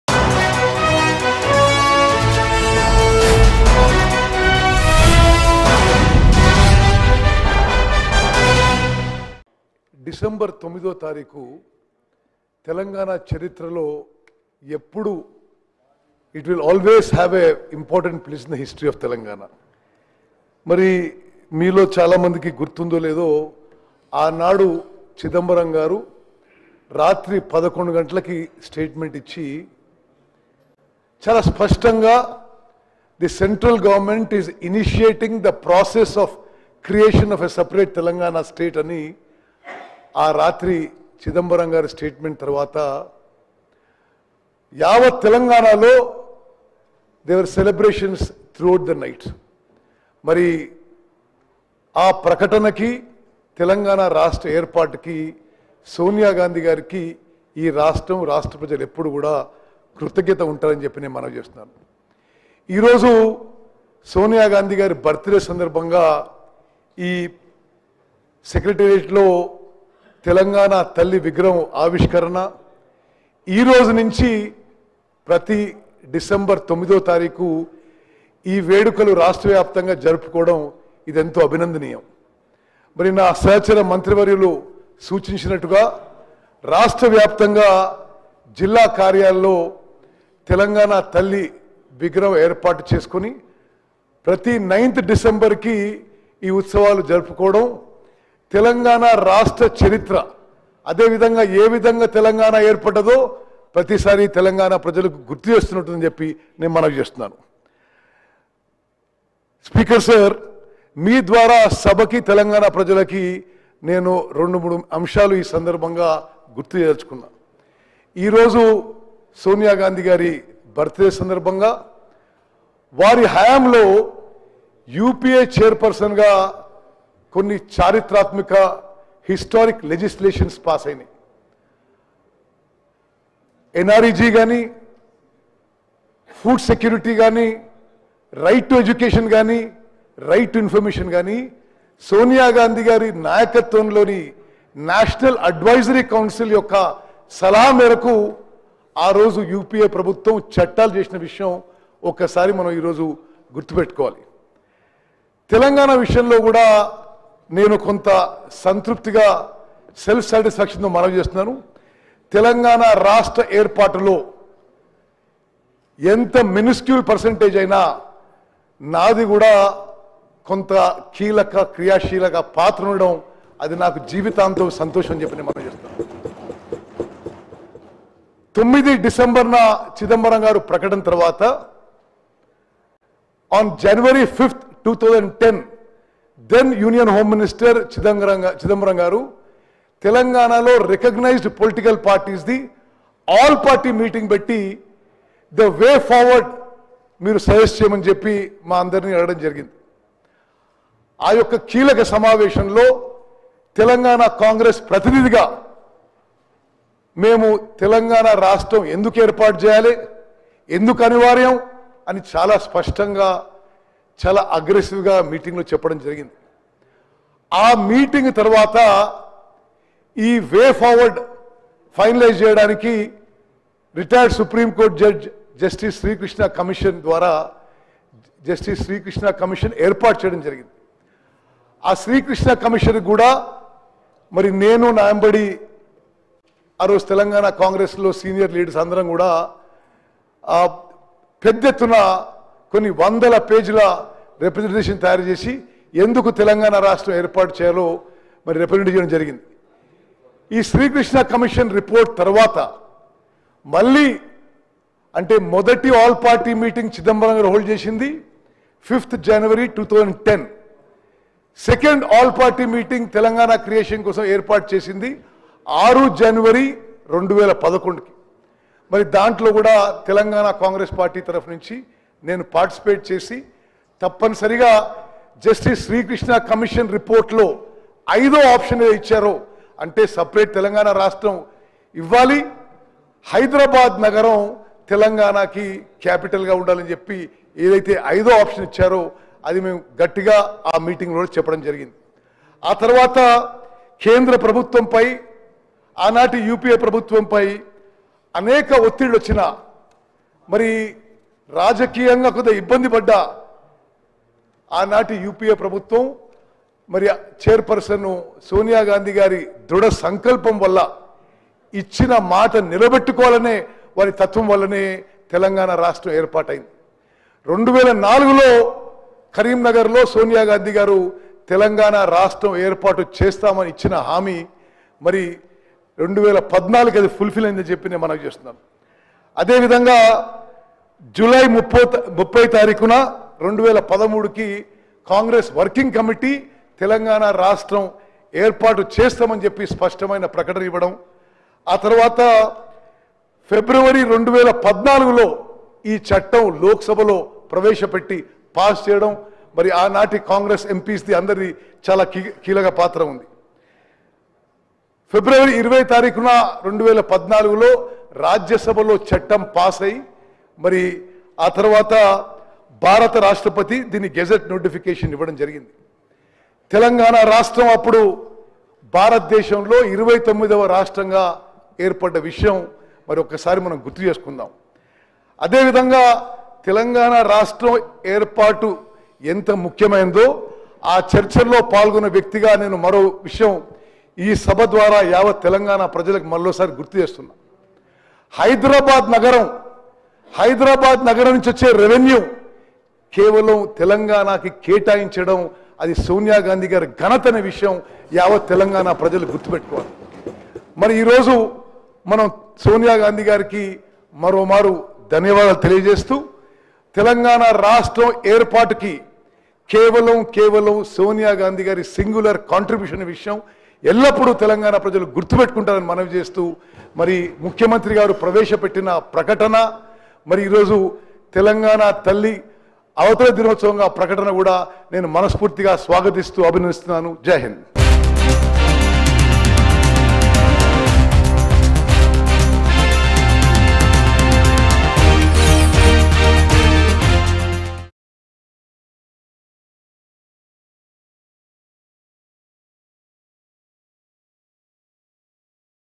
Minister Uttam Kumar Reddy Praises Sonia Gandhi in Telangana Assembly | Congress | Mango News
Minister Uttam Kumar Reddy Praises Sonia Gandhi in Telangana Assembly. The Telangana Assembly Winter Session Day 1. During this session, the government will introduce seven amendment bills, including the Panchayat Raj Bill and the Record of Rights Bill. Bills on employee salaries, pensions, and municipal reforms will also be presented. Intense debates between the ruling and opposition parties are expected.